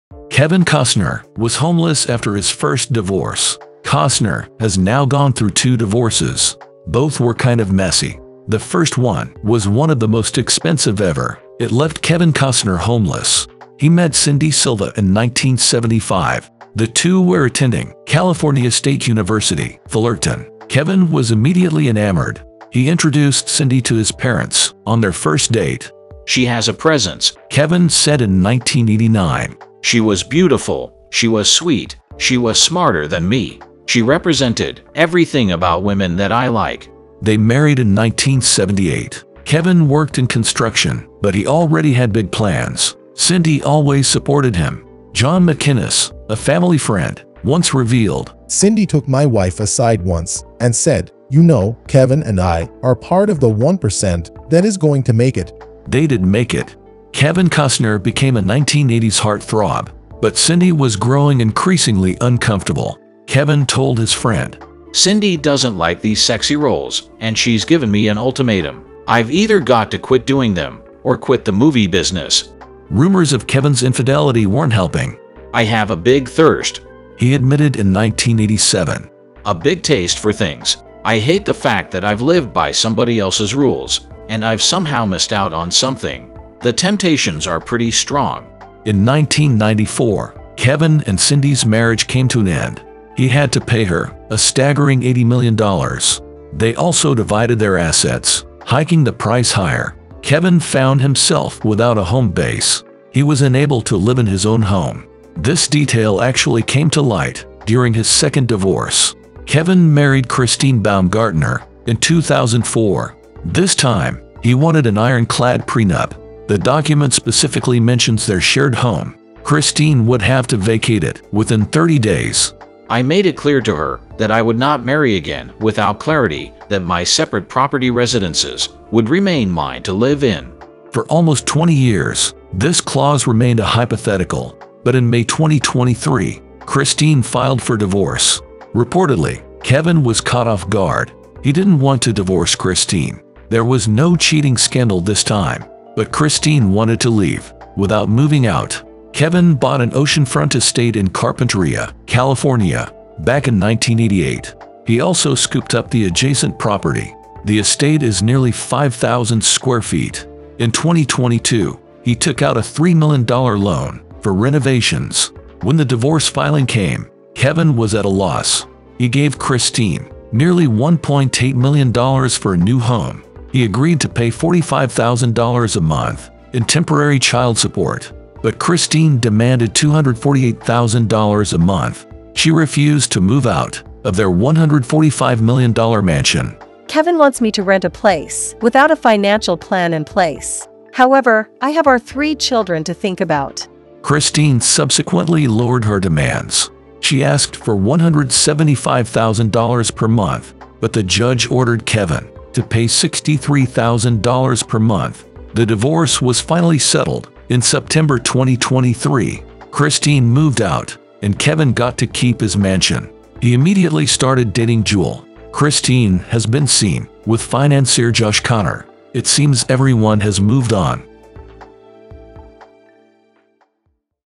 Kevin Costner Was Homeless After His First Divorce
Kevin Costner’s two marriages ended in messy divorce proceedings.